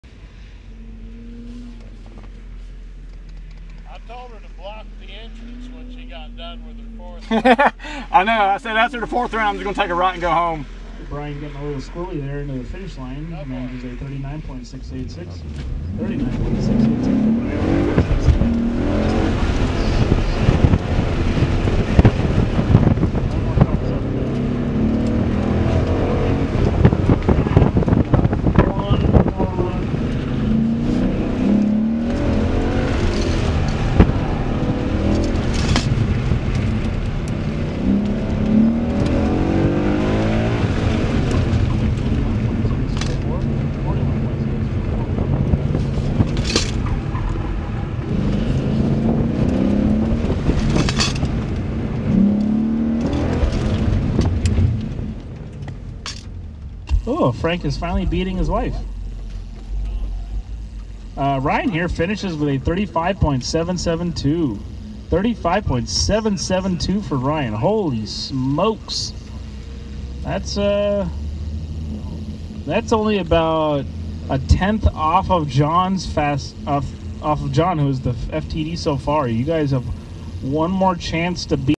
John's fastest lap Oct 20 2018
Switched cars with my friend John. I let him drive my Mustang GT and I drove his Mustang Ecoboost. Here is his fastest lap driving my car. The FTD ended up being high 34's or 35.0##, results have not been posted, yet. John's fastest time was 35.772 sec. I didn't drive my car this session, in the dry, so I'm not sure if I would have been faster or not. And I cannot remember my fastest time in his Ecoboost, I remember a 37.0, but I think my last lap was faster. Update when times are posted. (Also, john is over 6 feet tall, i'm 5'3" and my video camera is in a fixed position to the seat.  Unfortunately, I didn't think about that at the time and when John moved the seat down, the camera lost visual of the course.